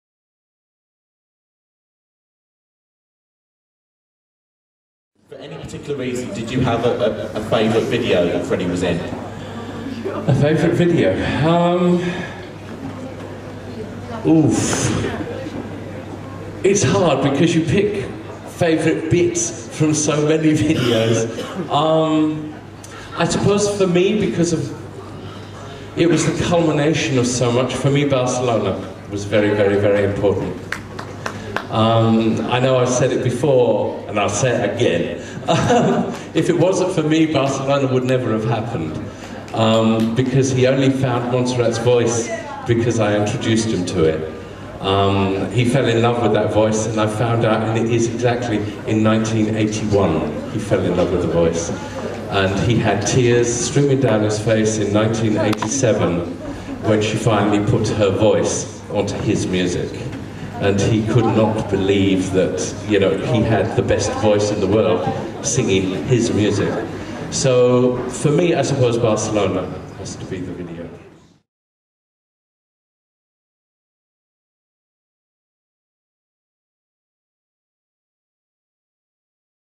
Peter Freestone Talks... About His Favorite Freddie Video
Peter Freestone, Freddie Mercury's former Personal Assistant, talks about Freddie at the 24th International Queen Convention March 29, 2009 at Ponton's Brean Sands, Somserset, England.

Meet Peter Freestone and see his presentation at this year's North American Queen Convention, July 12-14, 2013, in Toronto Canada.